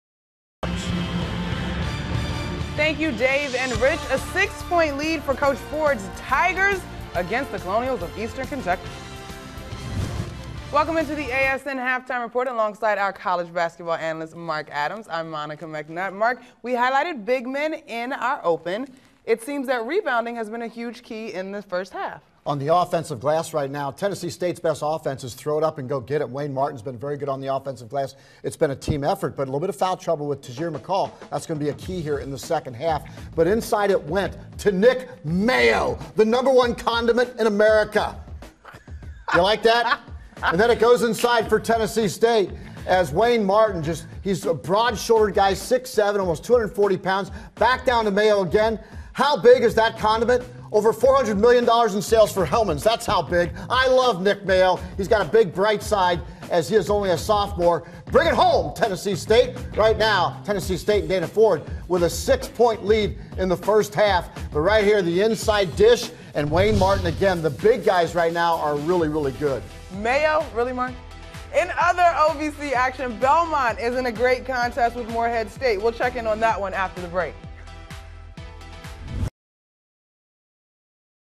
ASN Halftime Report: TSU vs. EKU
Monica McNutt and Mark Adams break down this Ohio Valley Conference battle between Tennessee State and Eastern Kentucky. TSU leads 32-26 at the half.